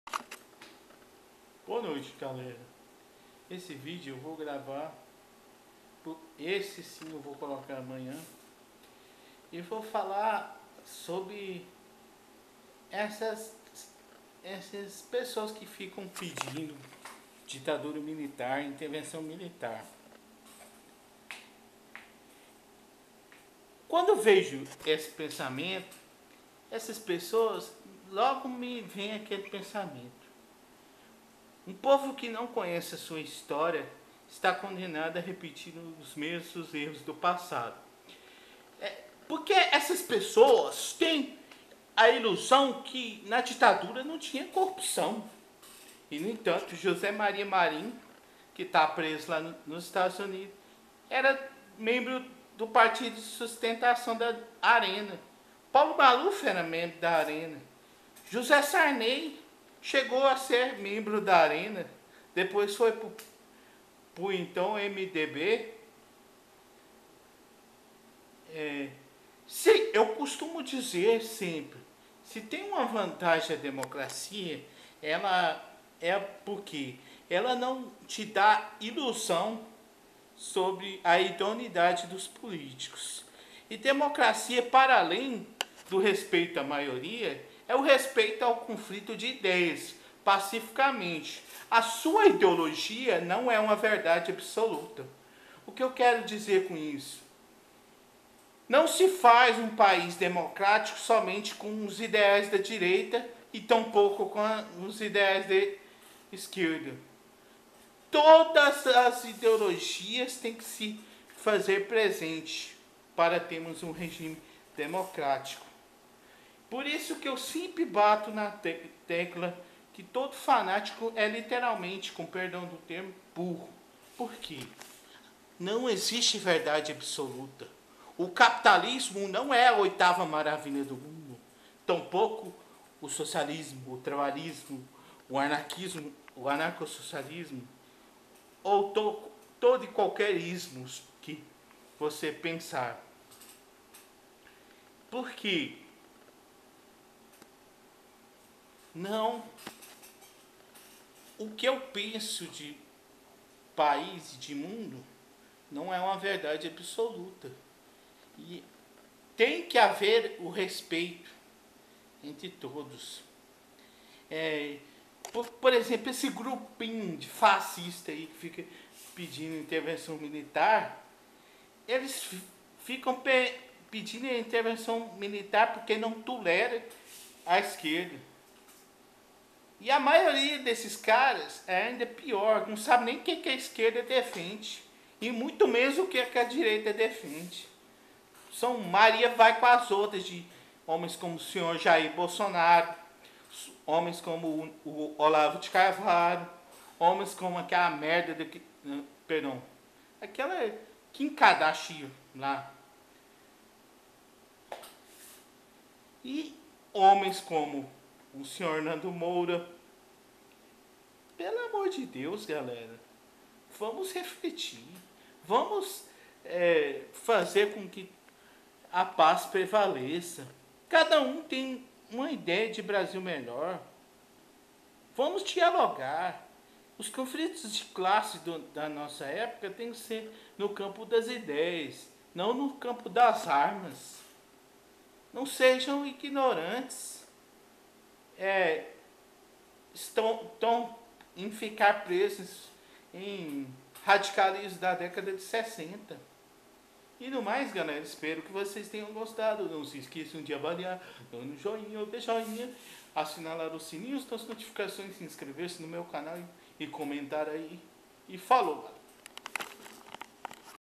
Mensagem para quem defende ditadura
Para os que defendem ditadura militar, eu digo que por mais defeito a democracia tenha, ela tem uma qualidade que a difere de qualquer ditadura ela não te da ilusão sobre a idoneidade da classe política em uma ditadura quem ousar denunciar é morto pela ditadura como ocorreu como Vladimir Herzog que já denunciava homens como Jose Maria marin e Maluf membros da Arena...

Pagina do Facebook oficial do meu canal